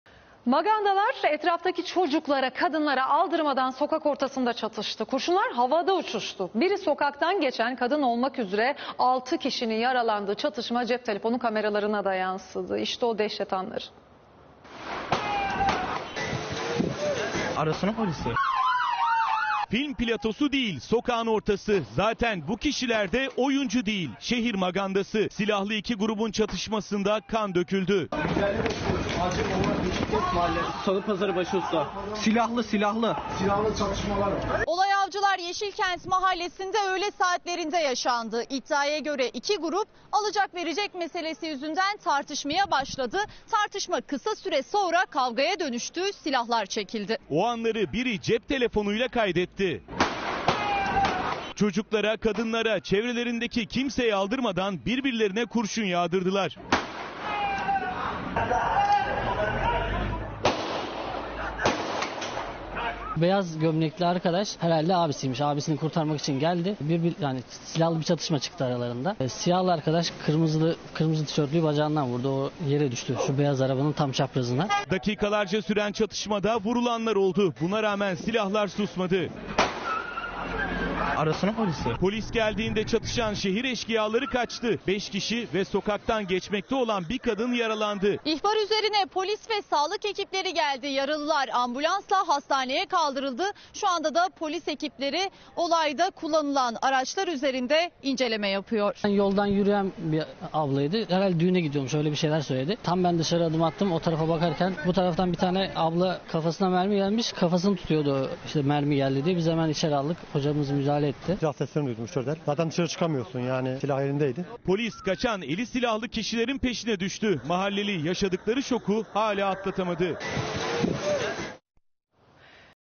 Sokak ortasında maganda çatışması!
MAGANDALAR,,, ETRAFTAKİ ÇOCUKLARA, KADINLARA ALDIRMADAN SOKAK ORTASINDA ÇATIŞTI... KURŞUNLAR HAVADA UÇUŞTU... BİRİ SOKAKTAN GEÇEN KADIN OLMAK ÜZERE 6 KİŞİNİN YARALANDIĞI ÇATIŞMA CEP TELEFONU KAMERASINA DA YANSIDI... İŞTE O DEHŞET ANLARI...